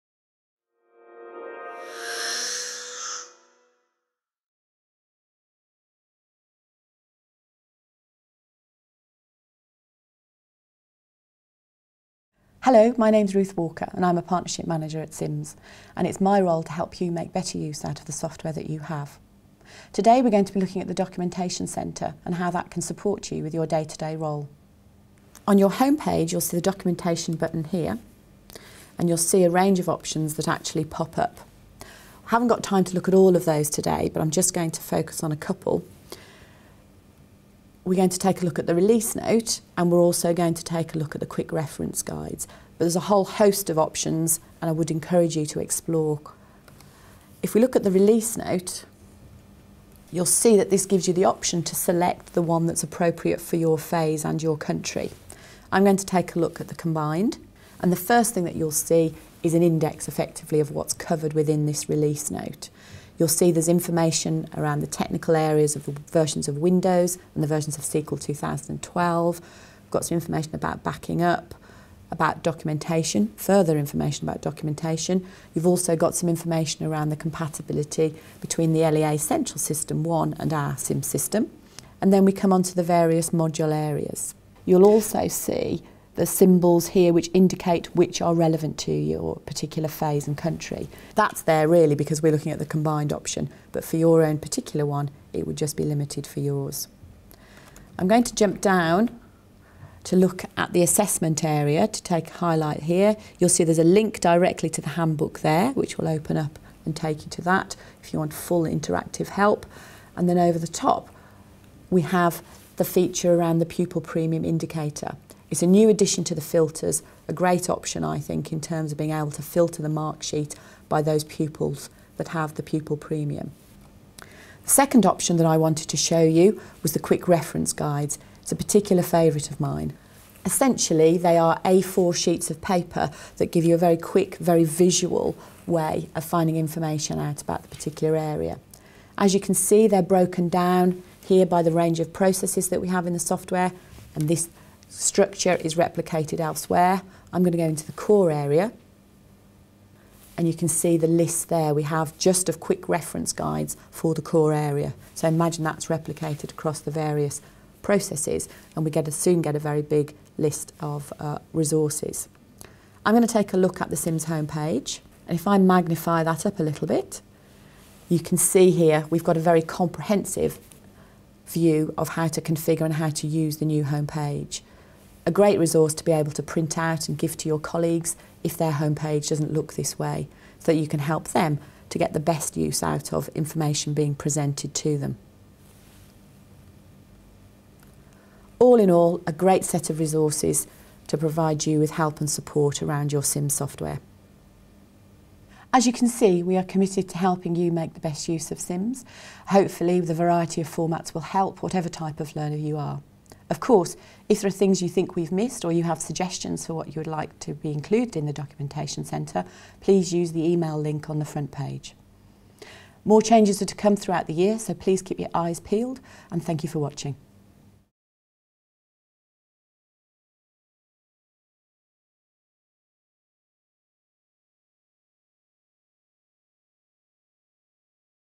Did you know the SIMS Documentation Centre can support your role?
Did you know the SIMS Documentation Centre can support your role? Partner Manager Ruth Walker shows you how.